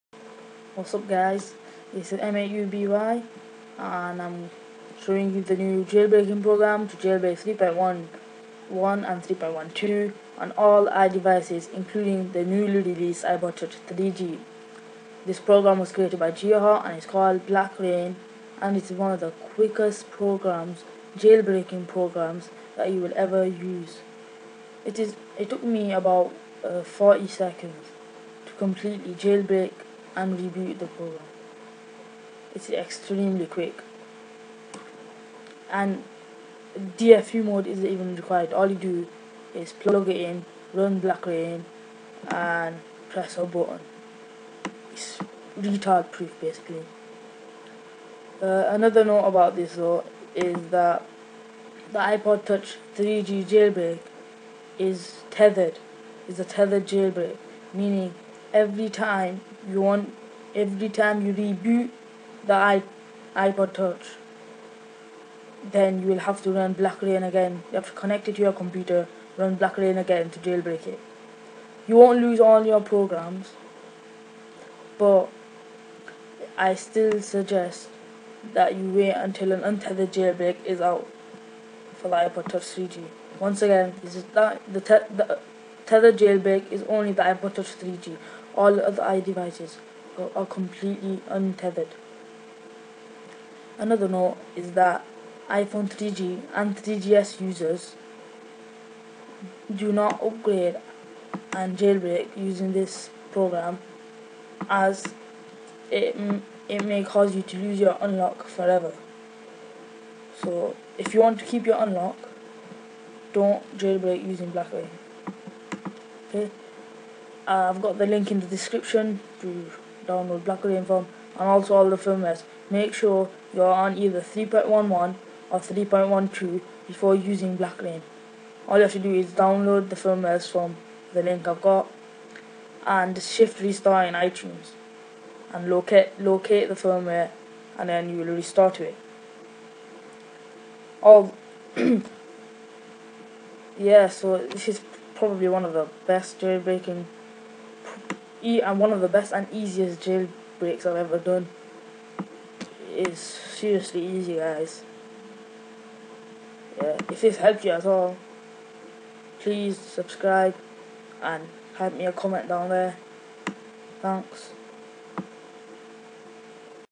Jailbreak 3.1.2 iPhone & iPod Touch w/ Blackra1n on Windows
In this video I will show you how to jailbreak 3.1.2 firmware iPhone and iPod Touch. ANY generation!

Notes:
- If you continue to have problems with this just wait for a jailbreak from the DevTeam. It will work perfectly with NO bugs.
- If your iPhone is Unlocked DO NOT do this guide, its only for a jailbreak.
- If you have problems with the program getting stuck on connect to itunes mode(recovery mode) close the program, reboot your device by hold home and power for 20 seconds and try the program again no matter what state your device is in.
- If you have an iPod Touch 3G (if it was bought after September 9th this is a tethered jailbreak. That means if you turn your device off and you want to turn it back on your have to plug it up to the computer and run Blackra1n again to get it to reboot.)
- If your new iPod Touch 2G/3G 8g model number starts with MB it'll work, if your model starts with MC it won't. Go to Settings - General - About to see your model number.
- If you get stuck at the part where it says "going into recovery mode" and it stops doing anything, manually put it in recovery mode. If you don't know how google it!
- Some Cydia applications aren't working at the moment. Just be patient they will update them!

Want to get free stuff.
PM me with your email.